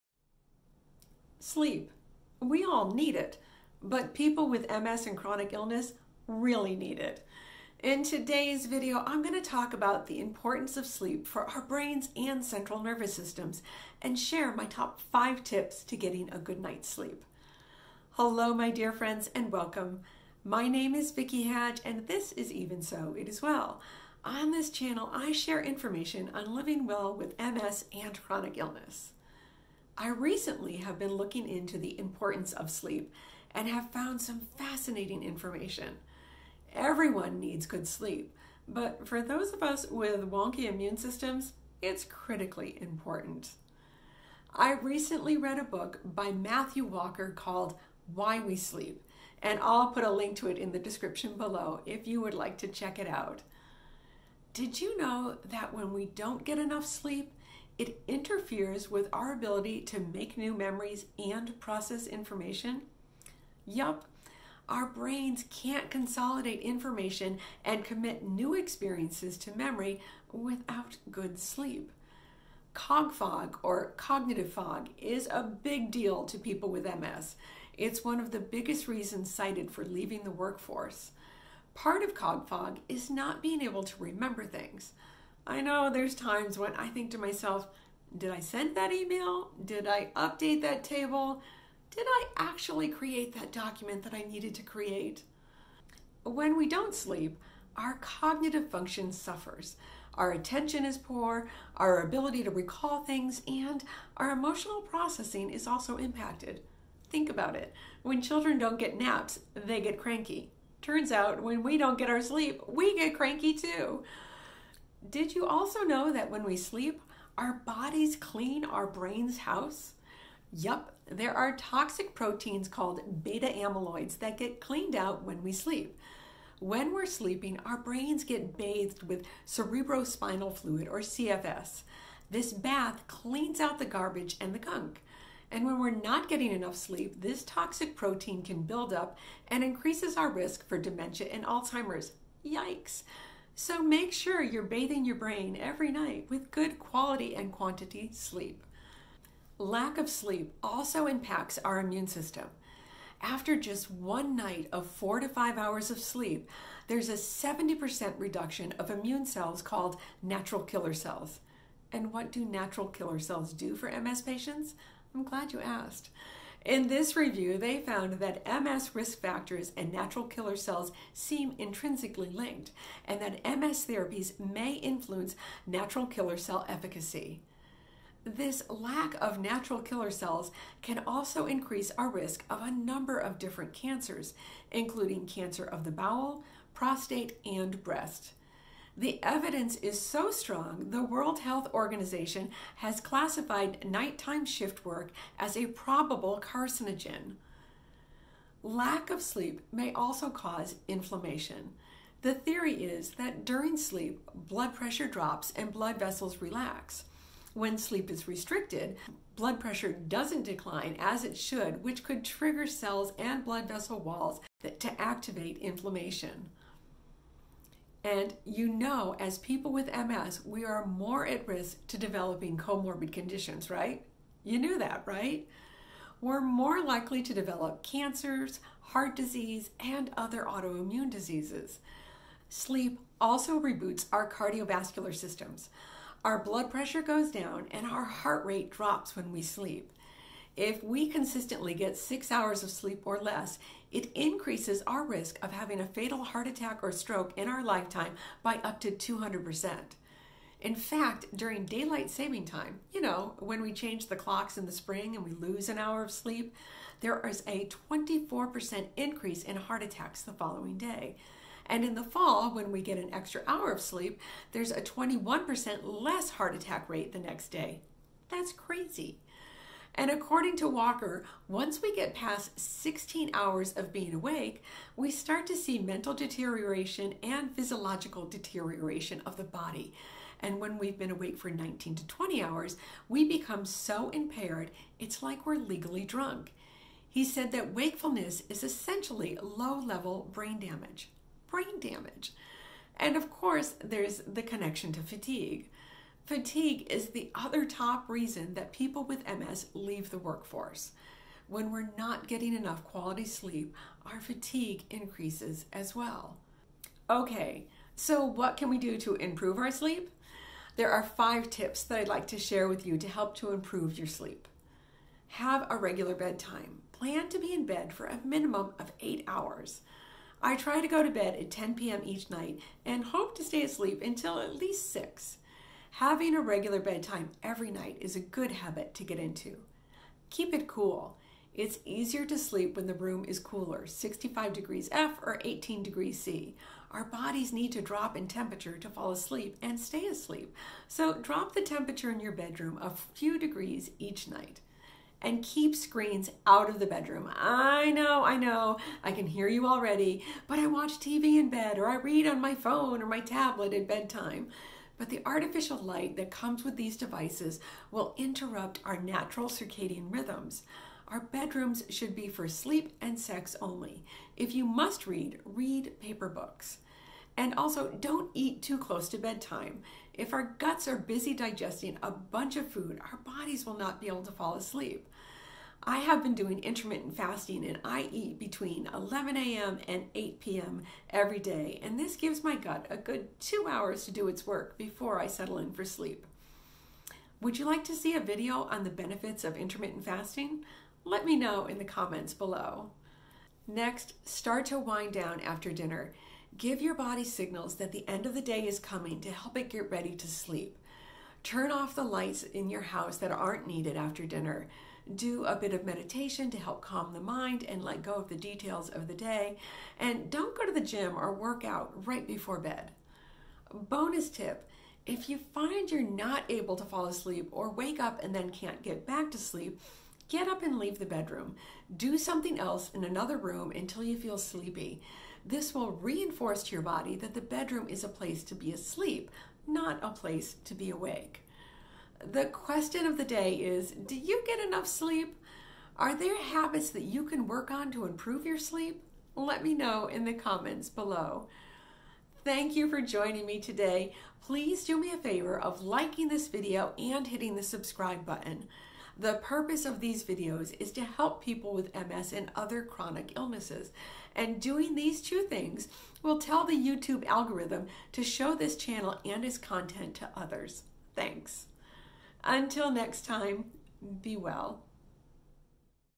MS and Sleep - Tips for Good Sleep
Sleep! We all need it and it is especially important to those with multiple sclerosis (MS) or chronic illness. I recently read Matthew Walker’s book Why We Sleep and have been looking into the importance of sleep. In this video I share information about how lack of sleep affects us including increased cog fog (cognitive fog) and fatigue. I also share my top 5 tips to getting a good night’s sleep.

** Why We Sleep by Matthew Walker **